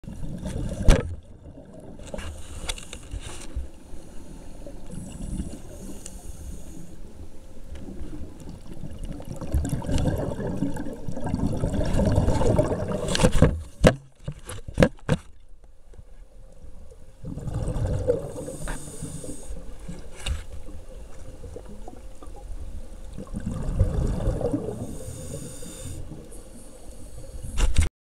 Tiger Beach - Lemons Get Jealous/Protective
Kind of like your dog when they see you petting another dog, huh? Bumping and pushing them away so they can hog all your attention. How can anyone think these guys are man eating monsters? Just look at their smiling faces.